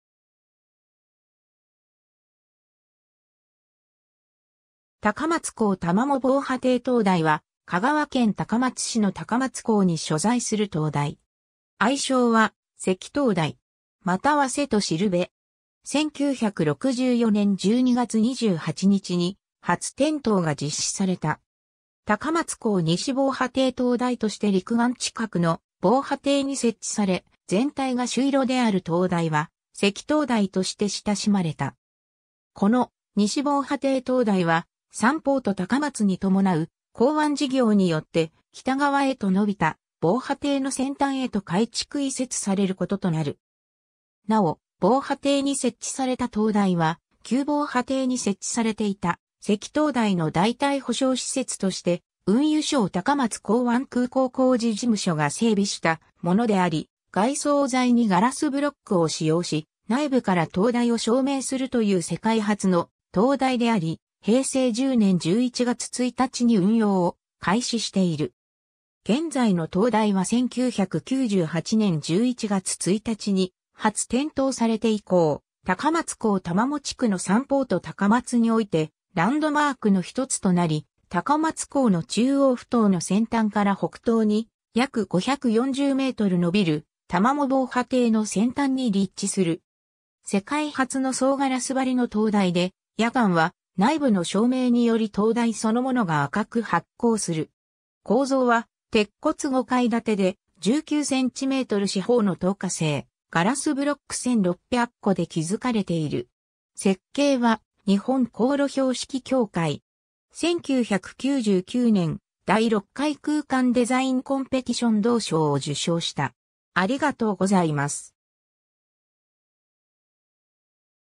高松港玉藻防波堤灯台
高松港玉藻防波堤灯台（たかまつこうたまもぼうはていとうだい）は香川県高松市の高松港に所在する灯台。
愛称は赤灯台。
またはせとしるべ。
 1964年（昭和39年）12月28日に初点灯が実施された。
高松港西防波堤灯台として陸岸近くの防波堤に設置され、全体が朱色である灯台は「赤灯台」として親しまれた。
この西防波堤灯台はサンポート高松に伴う港湾事業によって、北側へと伸びた防波堤の先端へと改築移設されることとなる。
 なお、防波堤に設置された灯台は、旧防波堤に設置されていた赤灯台の代替補償施設として、運輸省高松港湾空港工事事務所（現・国土交通省高松港湾空港整備事務所）が整備したものであり、外装材にガラスブロックを使用し、内部から灯台を照明するという世界初の灯台であり、平成10年11月1日に運用を開始している。
 現在の灯台は1998年（平成10年）11月1日に初点灯されて以降、高松港玉藻地区のサンポート高松においてランドマークの一つとなり、高松港の中央ふ頭の先端から北東に約540m延びる玉藻防波堤の先端に立地する。
世界初の総ガラス張りの灯台で、夜間は内部の照明により灯台そのものが赤く発光する。
構造は鉄骨5階建てで、19cm四方の透過性ガラスブロック1,600個で築かれている。
 設計は日本航路標識協会。
1999年、第6回空間デザインコンペティション銅賞を受賞した。